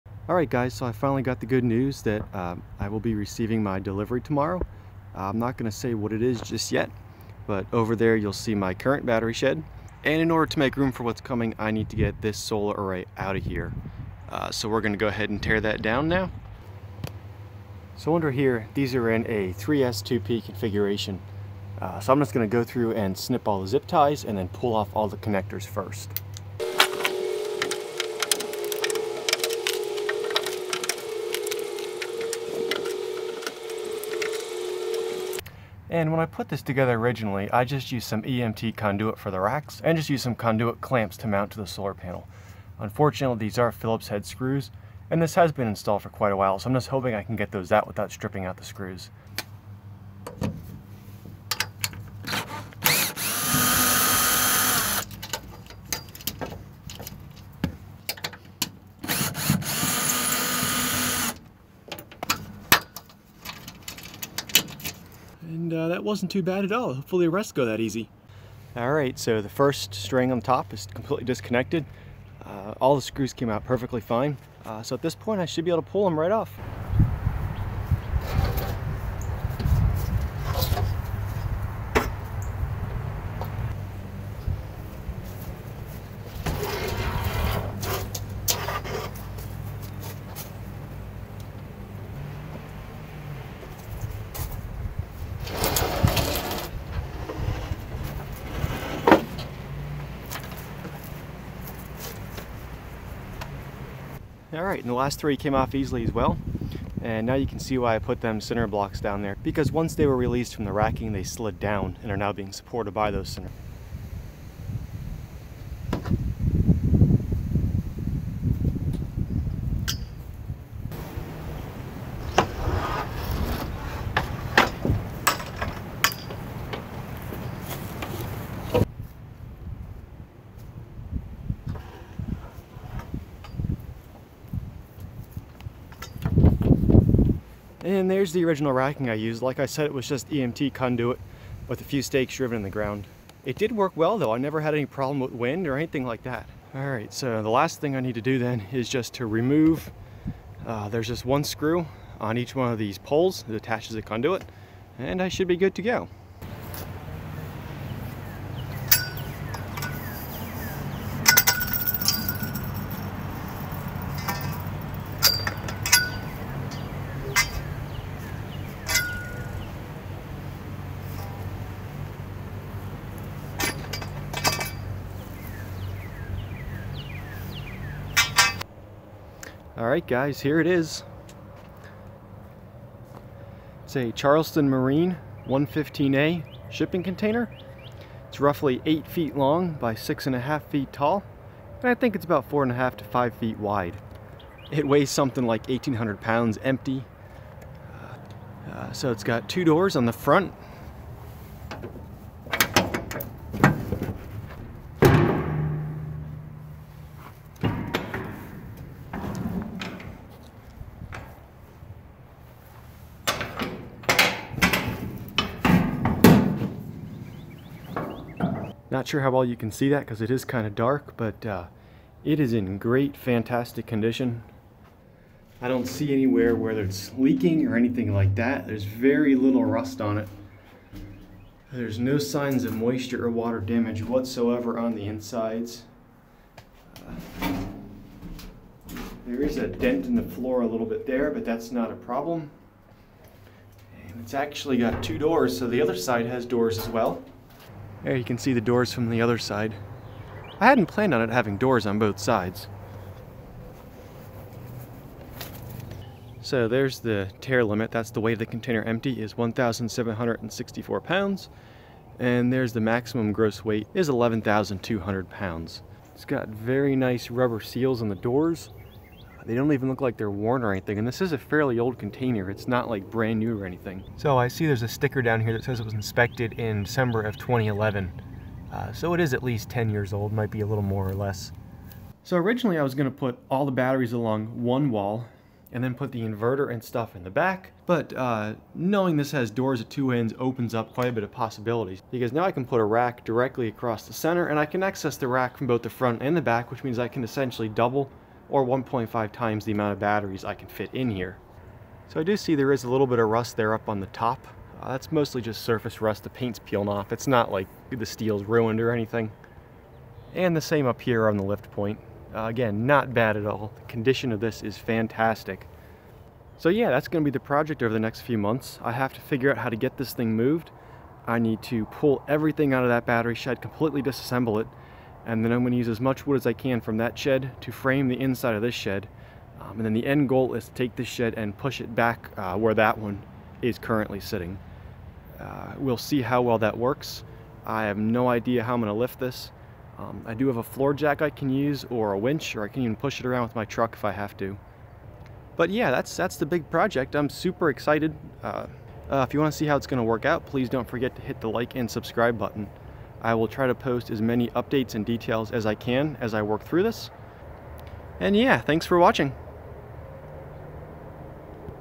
It's Upgrade Time! The Start of the Shipping Container Battery Shed
I am excited to announce the shipping container is finally here and can announce the "big project"! Over the next few months, I will be completely tearing down the current battery shed (even though it's not that old) and replacing with a steel shipping container, rebuilding everything as cleanly and code-compliant as possible. You won't want to miss this project!